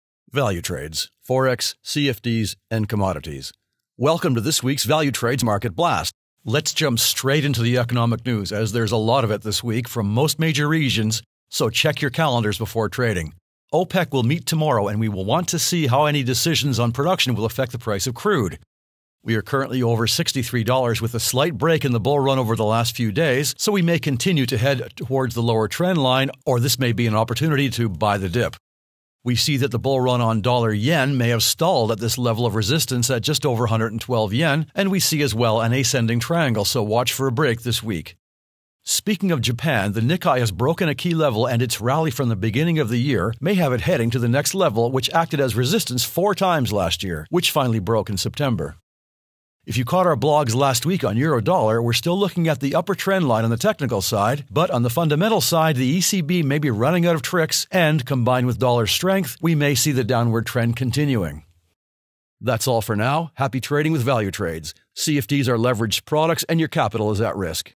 Market Blast 16 April 2019
See the latest trading news around EURUSD, USDJPY and JP225